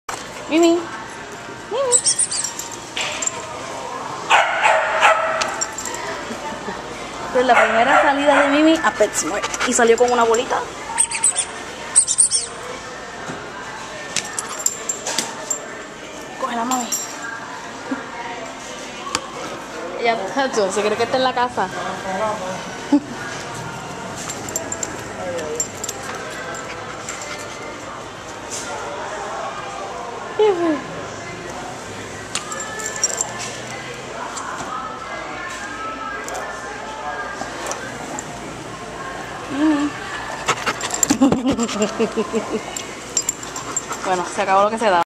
Princess Mimi en PetSmart
Primera vez que llevo a Mimi a PetSmart =)